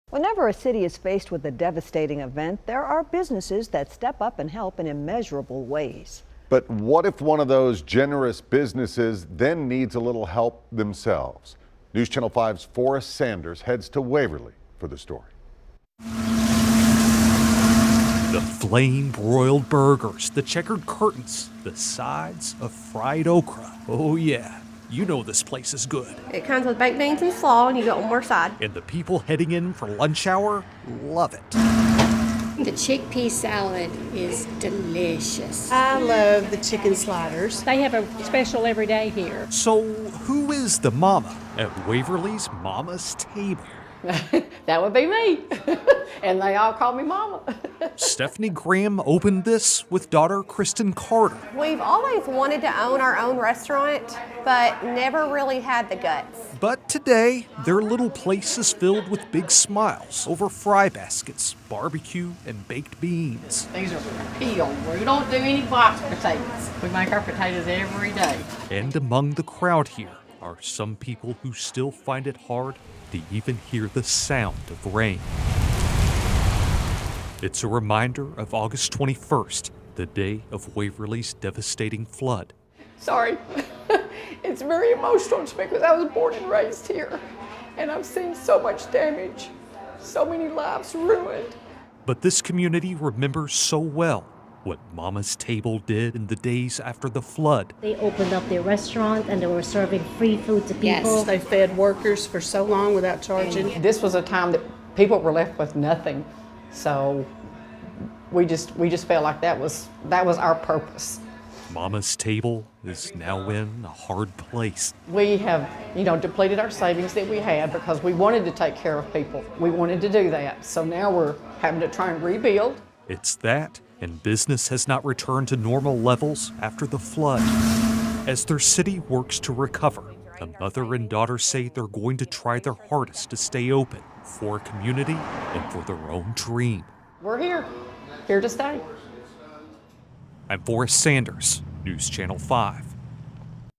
Waverly restaurant that helped with flood recovery now faces new business struggle
Whenever a city is faced with a devastating event, there are always businesses that step up and help in immeasurable ways. What if one of those generous businesses then needs a little help themselves?